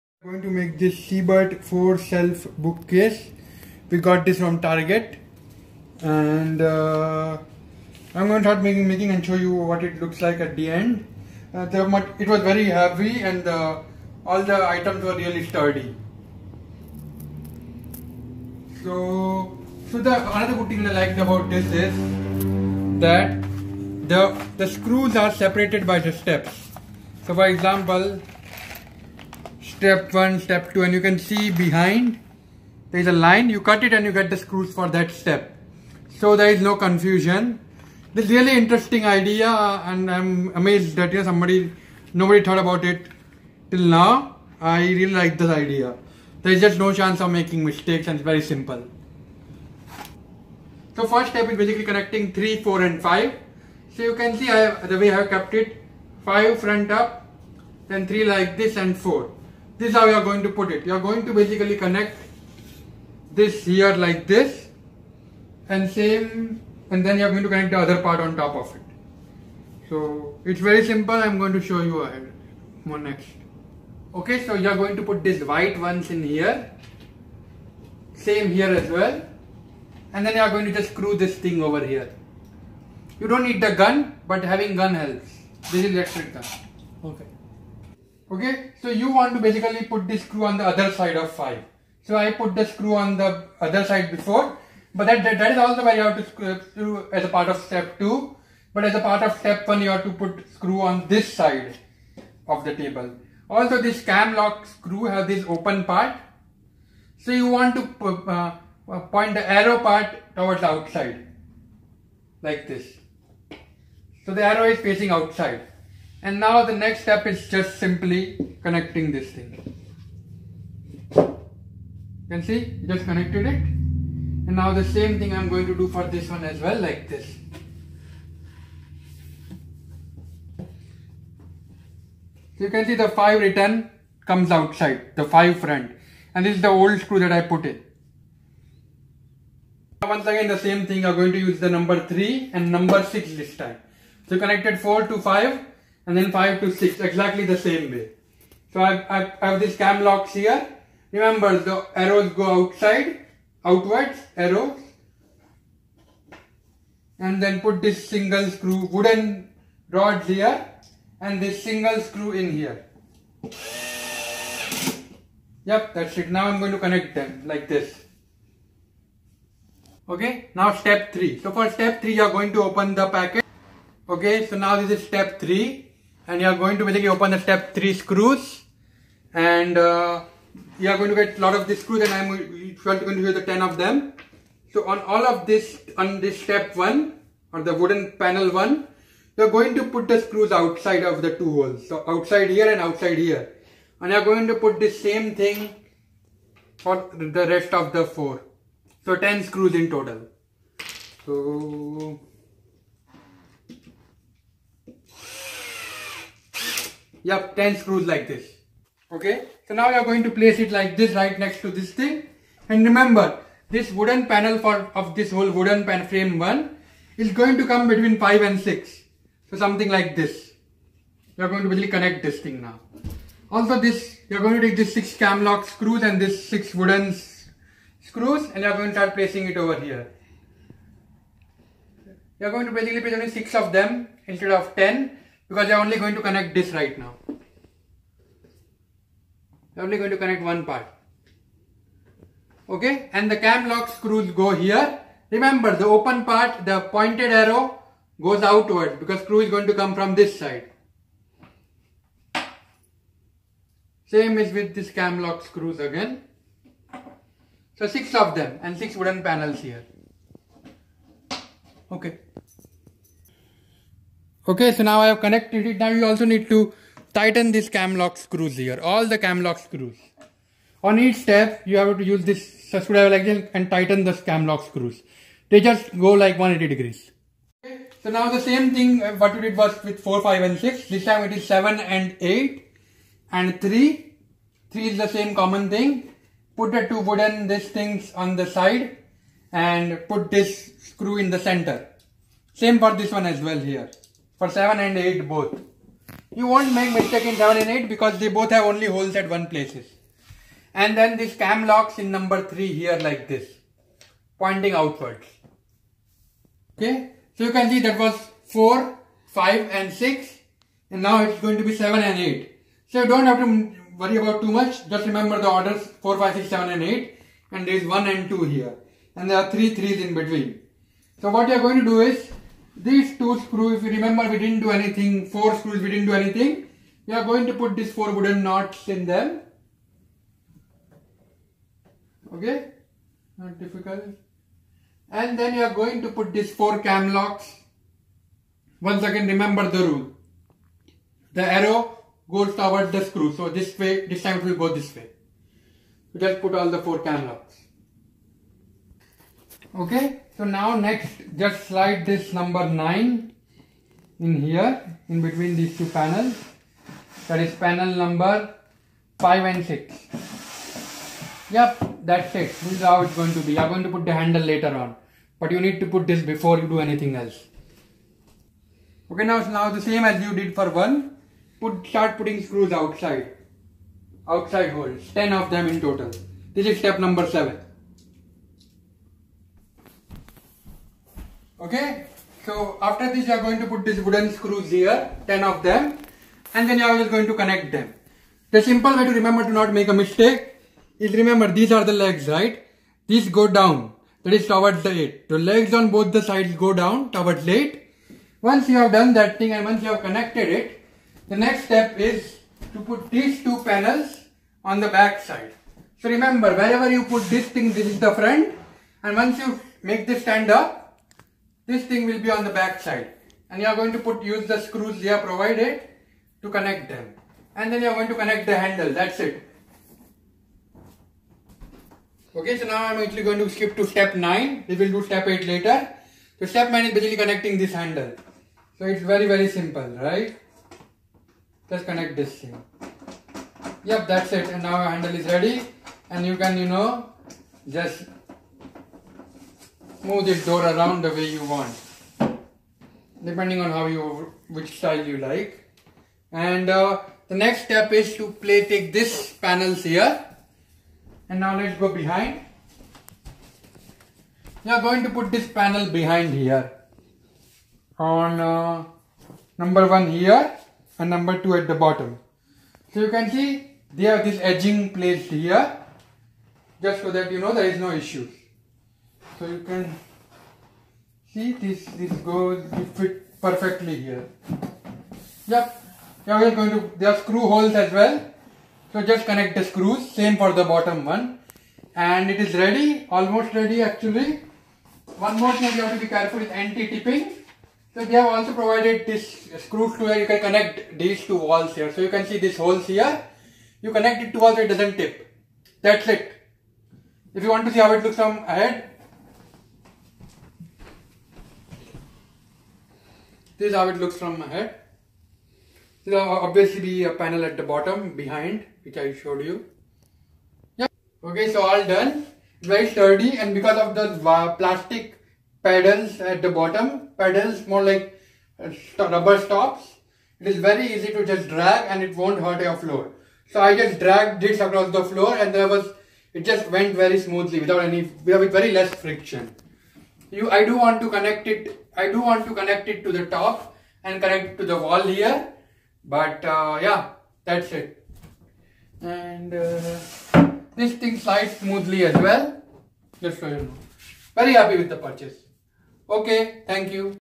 65.9" Siebert 4-Shelf Vertical Bookcase - Project 62™
Good design and style!
It looks great and is very sturdy. It has very unique design and also since the partitions are not symmetrical it looks very modern. Because it has two colors black and brown, i think it will look good with most home. After placing items in it, it looks even better. Also they have provided extra screws as well as stickers to hide the screws, which showed the attention to detail that was put in. Also the small rubber legs at the bottom are very smooth and makes moving the shelf very easy without causing any damages to the floor.

Installation was easy thanks to the way screws were packaged. Screws were separated by each step instead of by type, which made the installation very easy. You just removed the screw needed for that step while building which just got rid of confusion. I am very happy with it and would recommend this item.